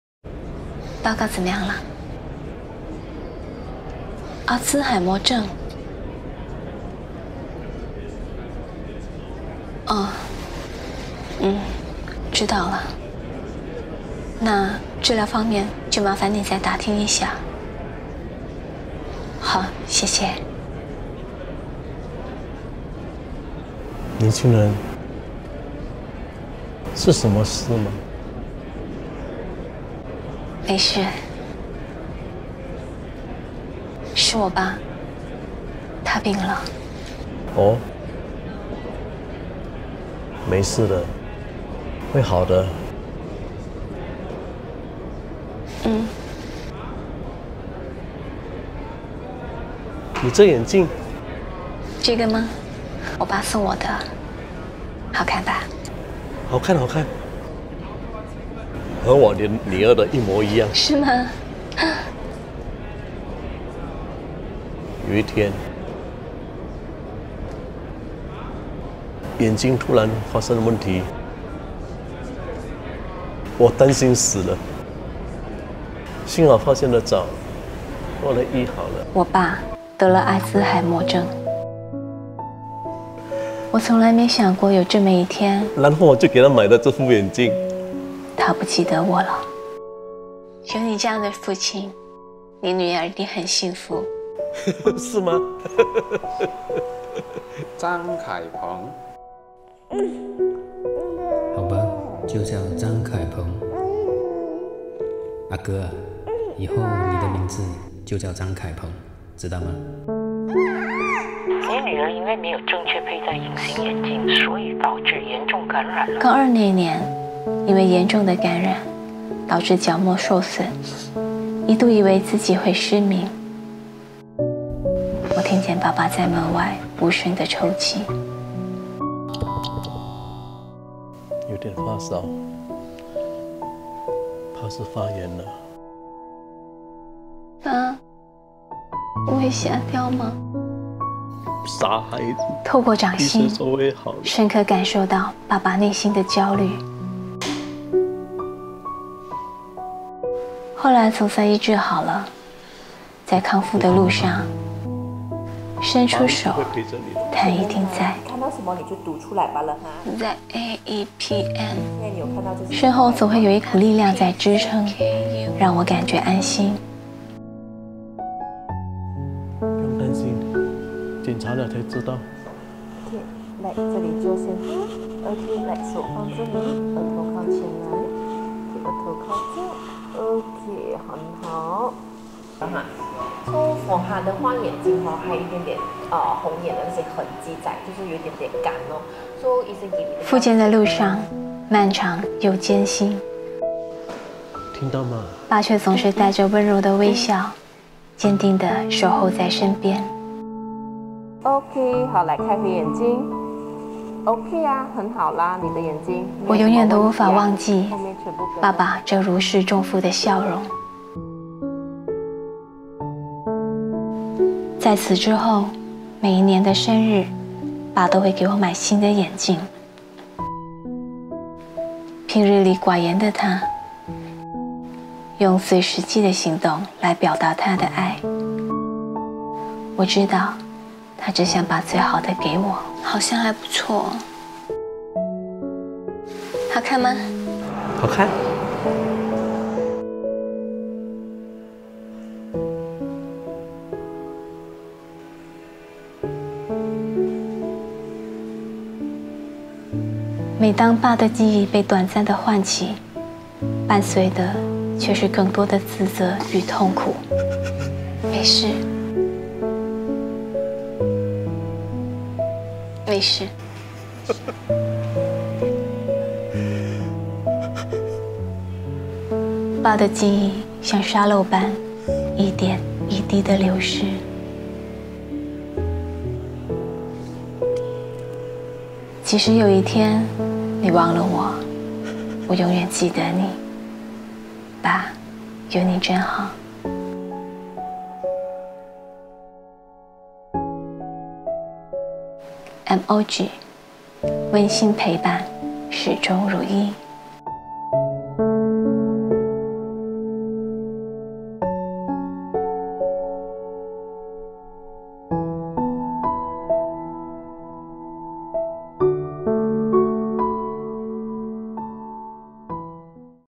MOG 温馨陪伴, 始终如一.
【爸，有你真好 👨‍👧 】
”即使有一天你忘了我，这份爱，我永远铭记在心” ♾

爸爸永远是孩子们第一个偶像，他总是你最棒的靠山，最可依赖的肩旁，让你看透世界最美好的一面。👨🏻‍🍼

我们一年一年的长大，他也一年一年的老去。👴🏼 今后，让我们成为他的依靠，牵着他度过睛彩的晚年。

让MOG与你从心出发，珍惜眼前人。祝大家能和爸爸度过个美满的父亲节 🥰